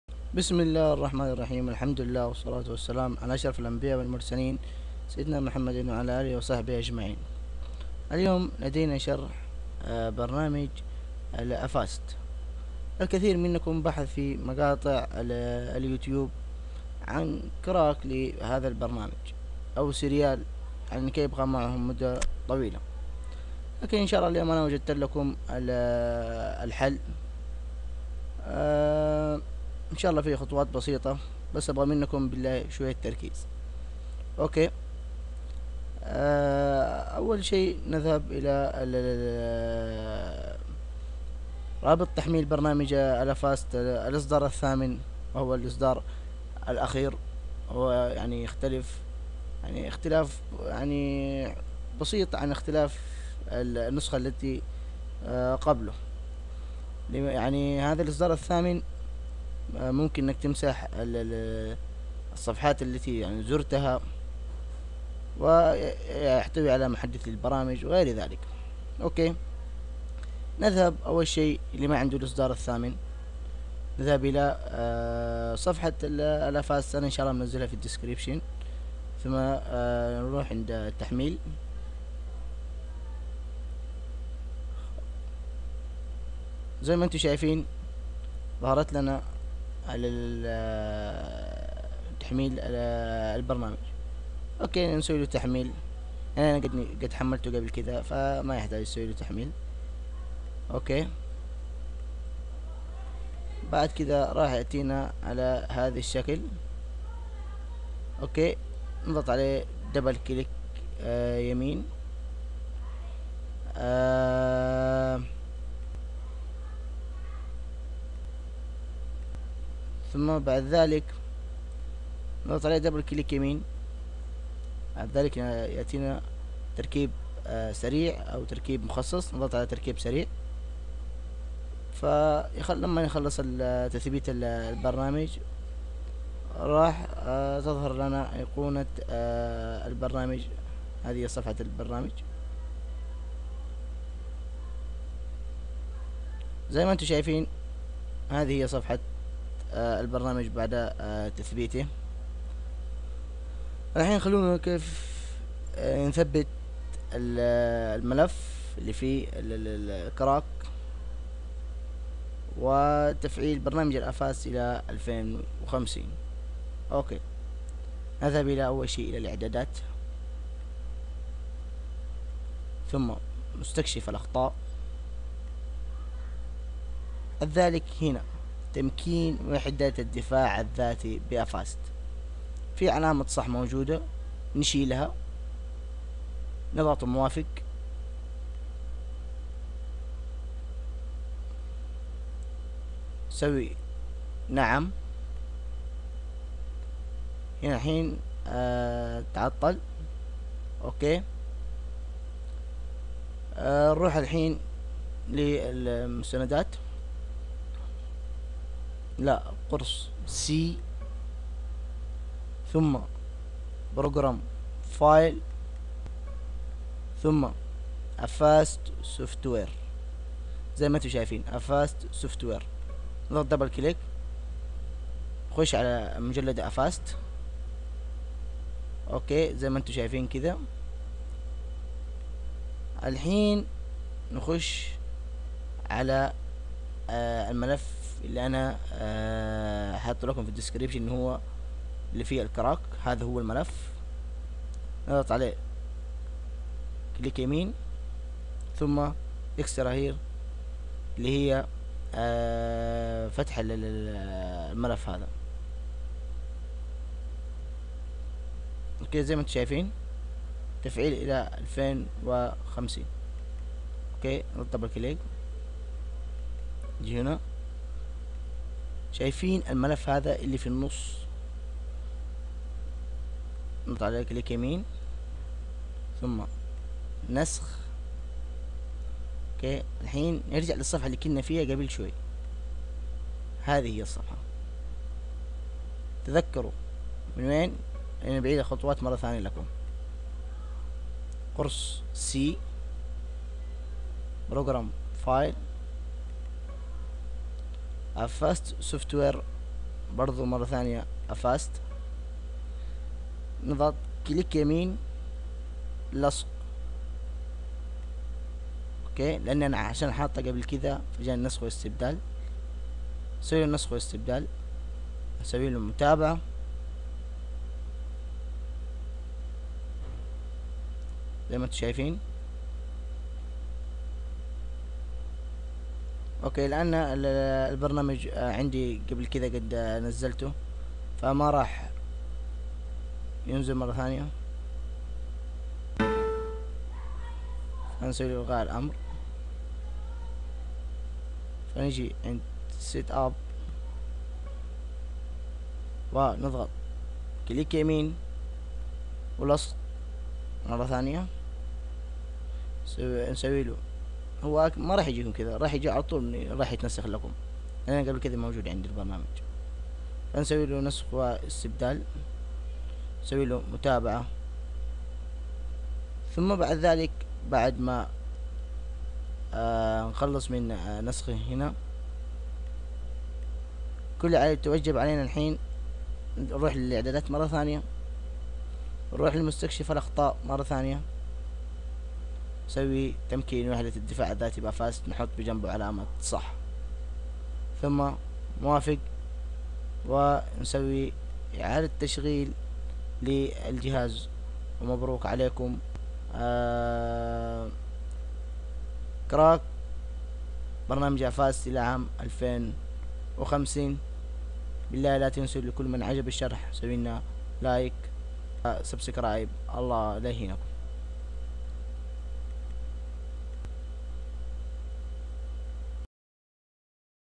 شرح تفعيل برنامج الافاست الى 2050مجانا جديد الحقوا HD
عفوا اخواني رابط الكراك لايعمل لانه تم اقفاله من الميديا فاير ولاكن جبتلكم سيريال
C2092321R9946A0912-U52DEWK3
تحطو في استماره التسجيل بعد انتهاء المده التجريببيه
رابط تحميل برنامجلافاست8
مع تحياتMR.GENERAL.KHALO0ODI